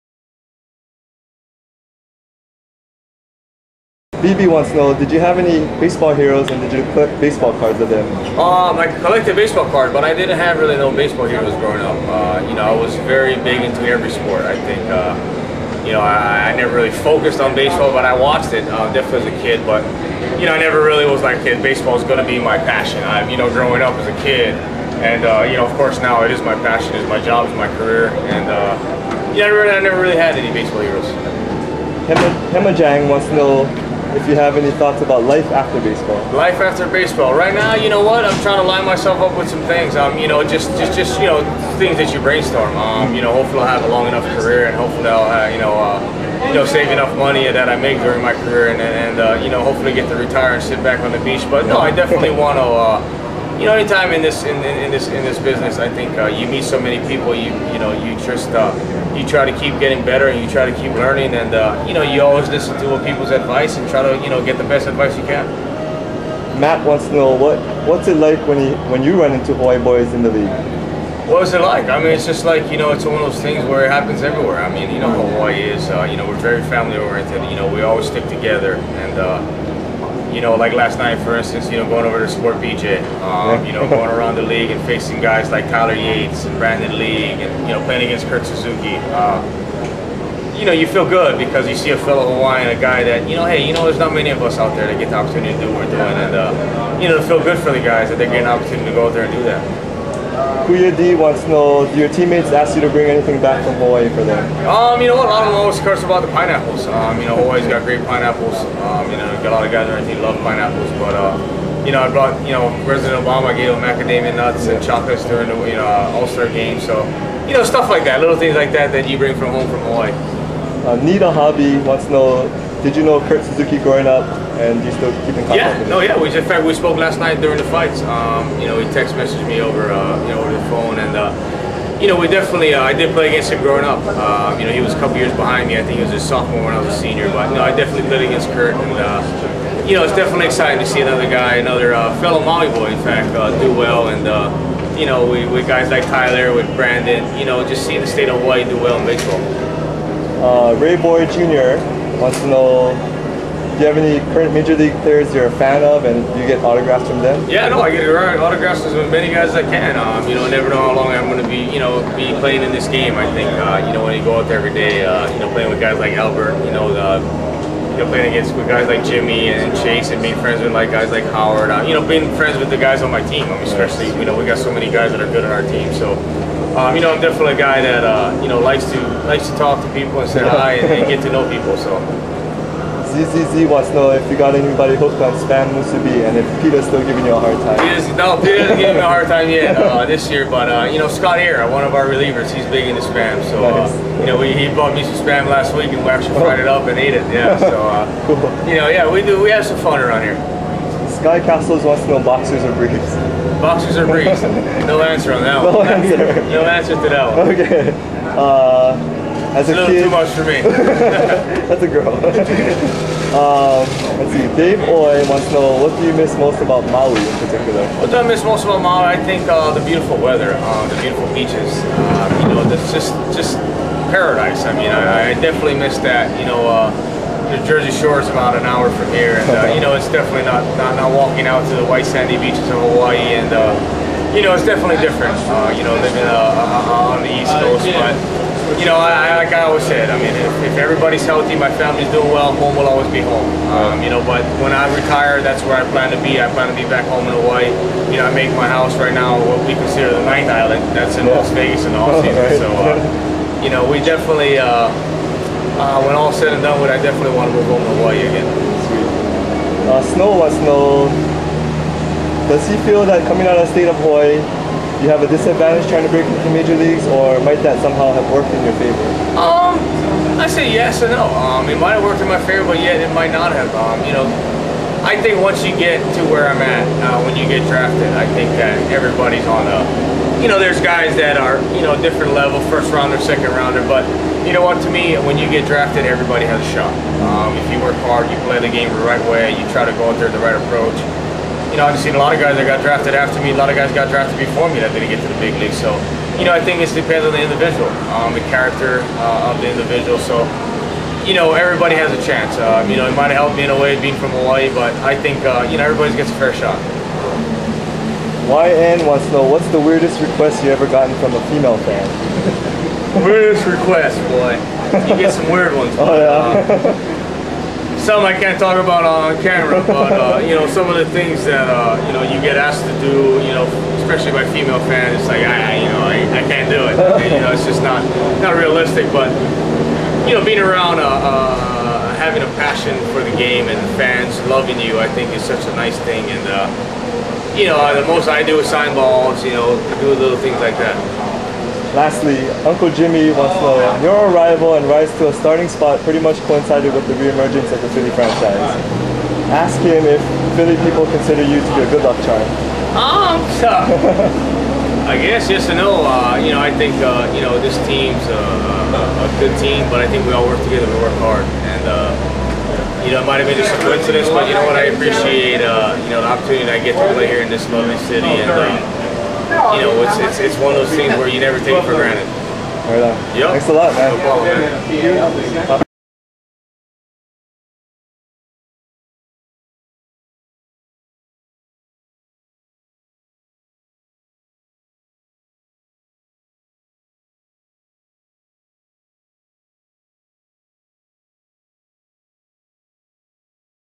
Shane Victorino Q & A
Philadelphia Phillies' All Star Centerfielder takes questions from Edward Sugimoto's World Wide Ed blog Community.

For the complete article and interview, please